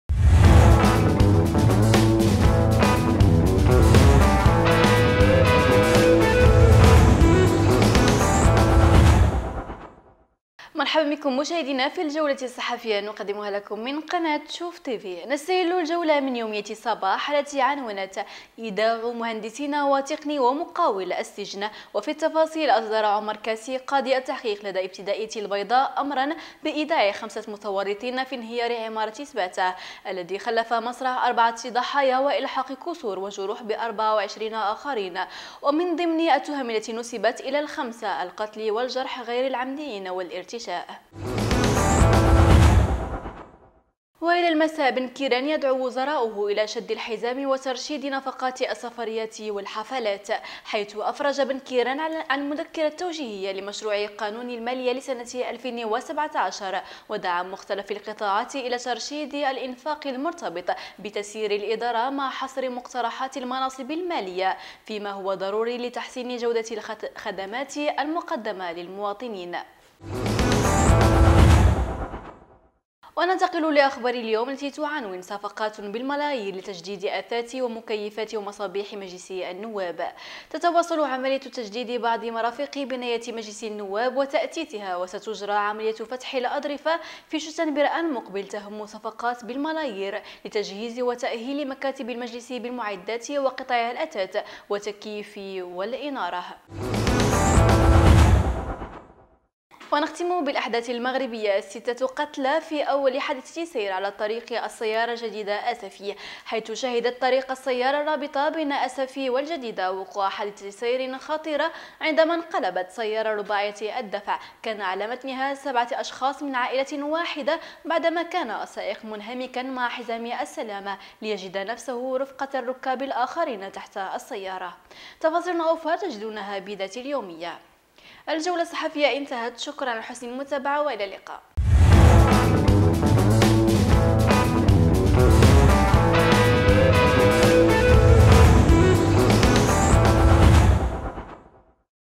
شوف الصحافة : بنكيران يدعو وزراءه إلى شد الحزام وترشيد نفقات السفريات والحفلات‎ | شوف تيفي
شوف الصحافة : بنكيران يدعو وزراءه إلى شد الحزام وترشيد نفقات السفريات والحفلات‎ | شوف تيفي 2016